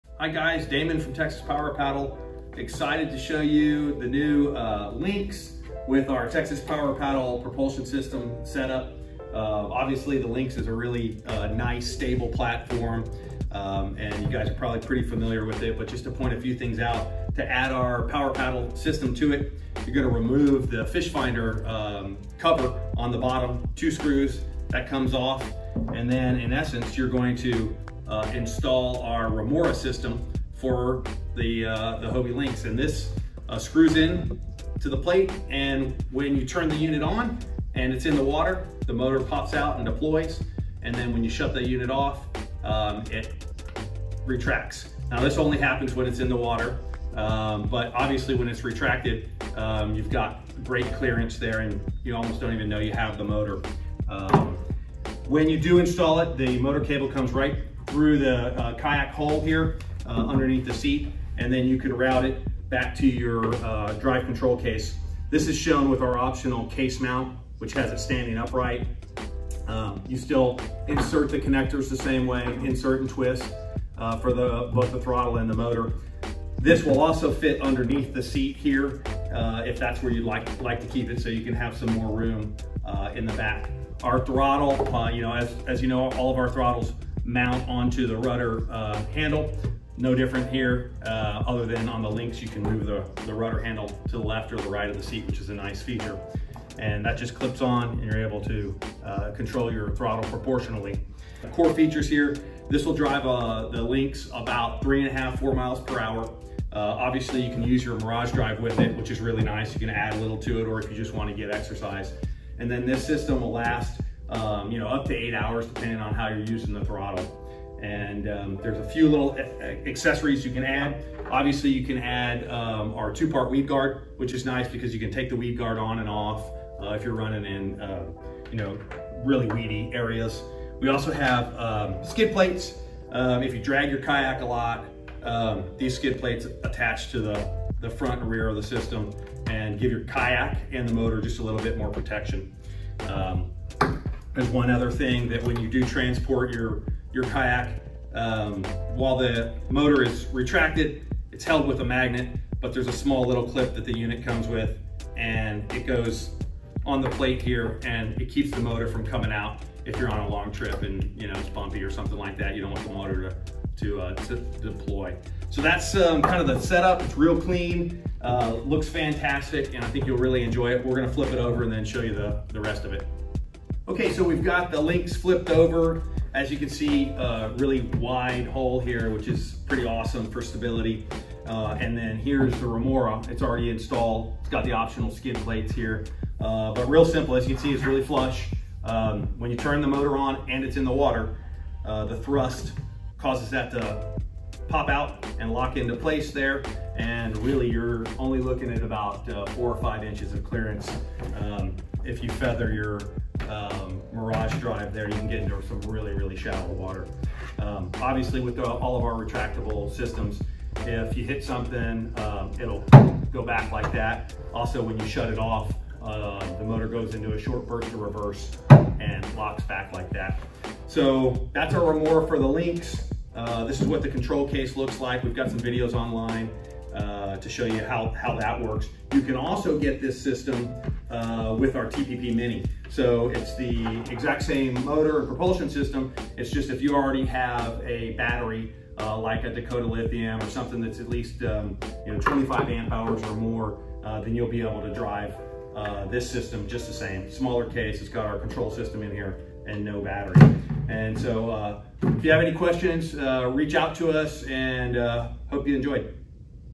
Hobie Lynx Remora Install video
In this video Damon goes over our REMORA unit outfitted in a Hobie Lynx. He will go over everything you need to know about the Remora including the install process, its capabilities, and accessories.